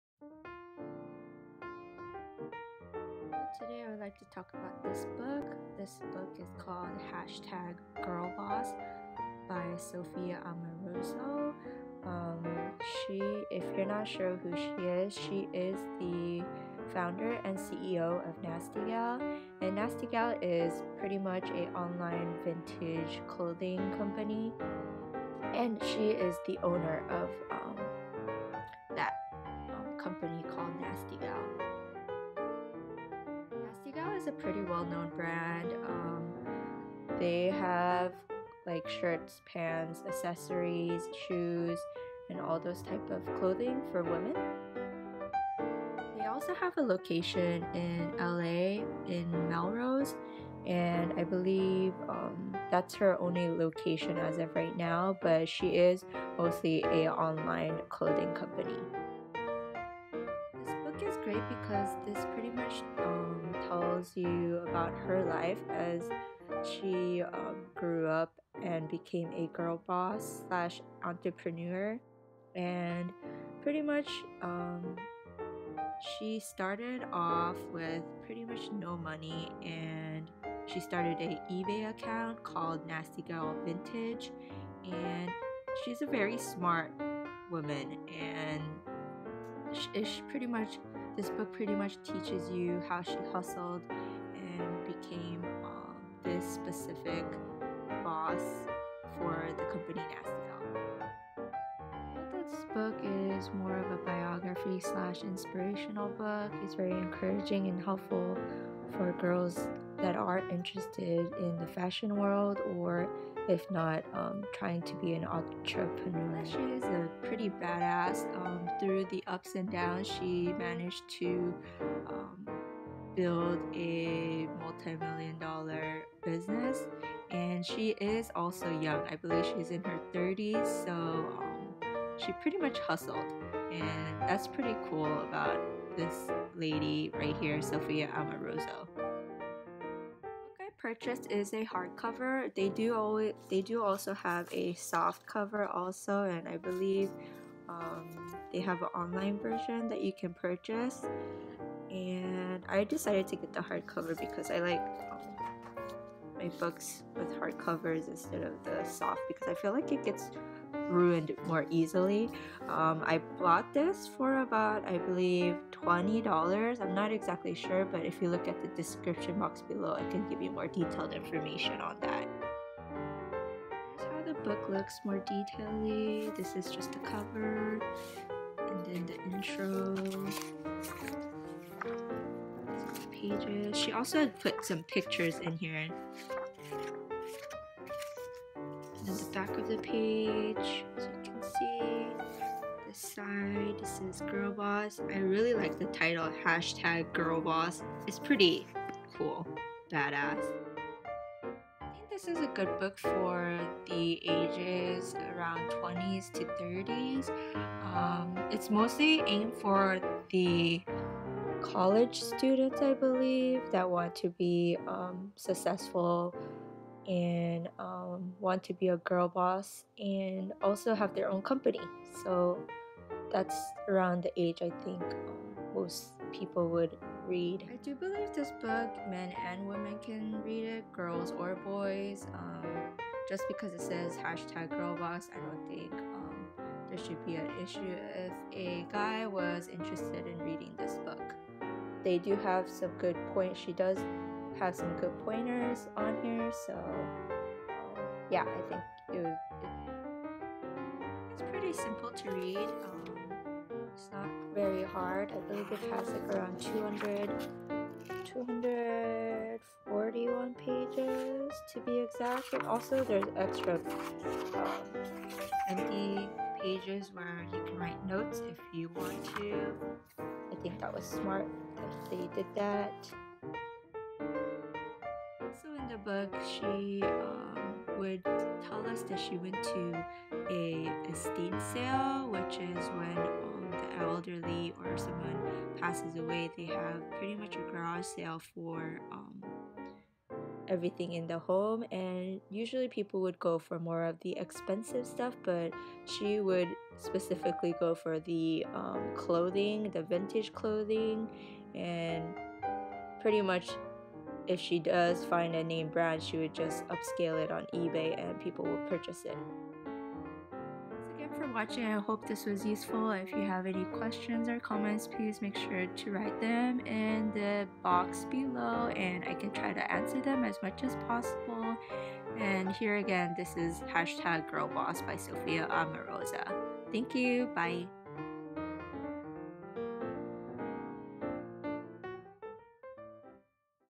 ✅ GIRLBOSS Book Review
Price Check

GIRLBOSS Book

Meet The New York Times Best Seller GIRLBOSS, now in paperback with a new introduction. The GIRLBOSS paperback is perfect for traveling light, thinking heavy, and giving to your most badass friends. The first thing Sophia Amoruso sold online wasn’t fashion—it was a stolen book. She spent her teens hitchhiking, committing petty theft, and dumpster diving.

By twenty-two, Sophia had resigned herself to working a mediocre day job she’d taken for the health insurance. It was there that she decided to start selling vintage clothes on eBay. Eight years later, she is the Founder & Executive Chairman of Nasty Gal. GIRLBOSS includes Sophia’s story, yet is infinitely bigger than Sophia.